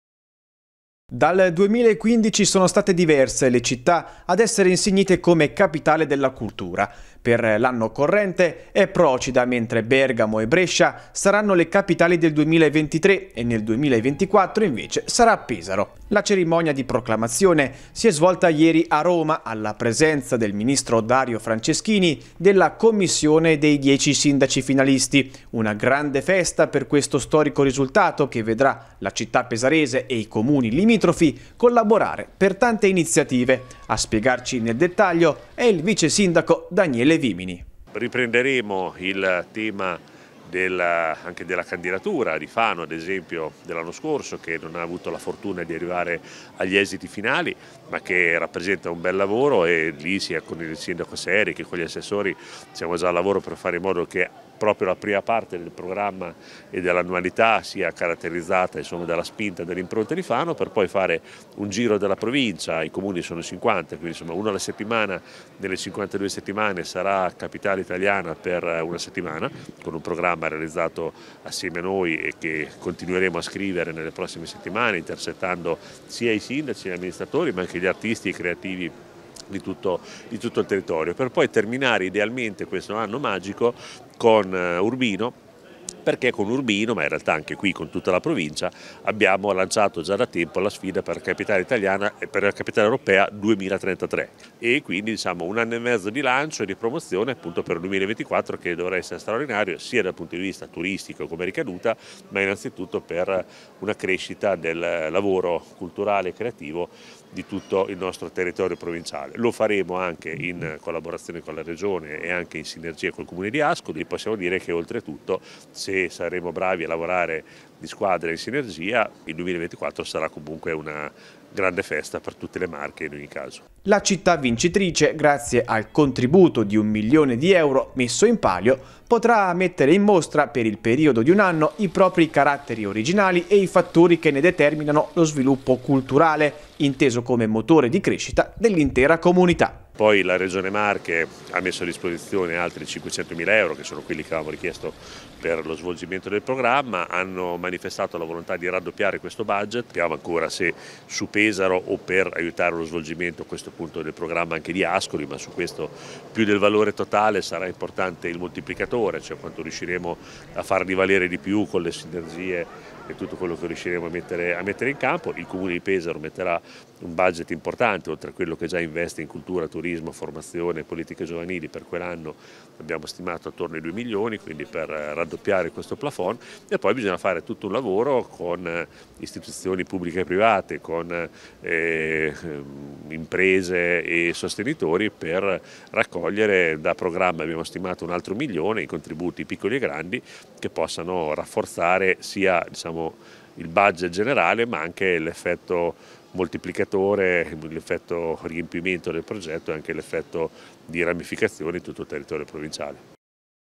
Pesaro, Capitale Cultura 2024: iniziative e contributi
Pesaro è stata proclamata Capitale della Cultura Italiana per il 2024. Ma quali sono le iniziative in programma e i contributi che spettano alla città pesarese?